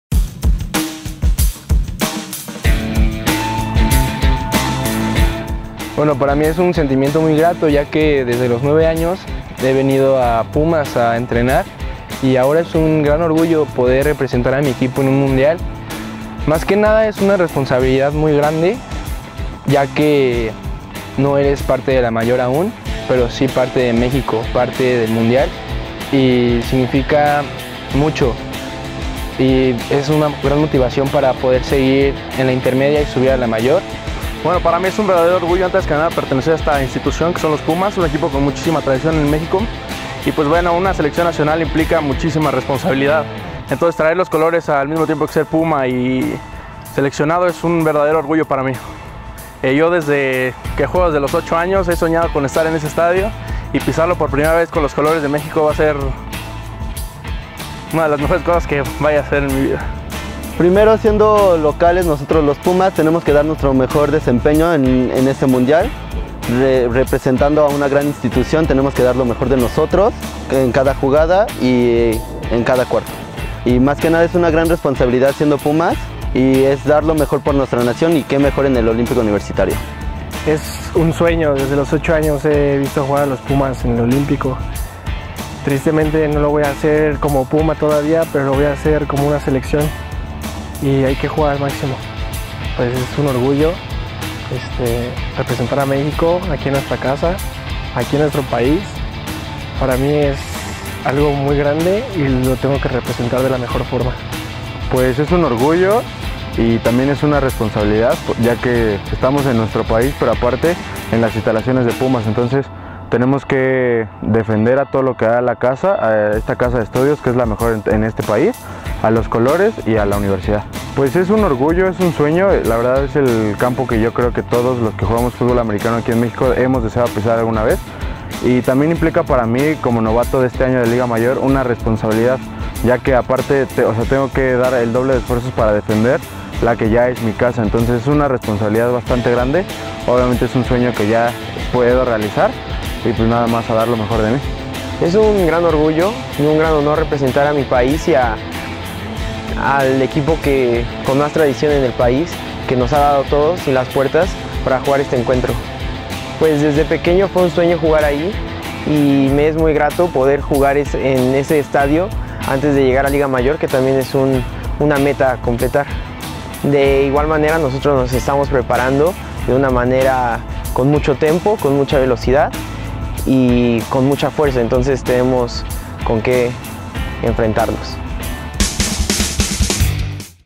Pumas seleccionados para el Mundial U19
Ellos son algunos de los Pumas seleccionados para representar a México en el Mundial U19 de la IFAF en 2018.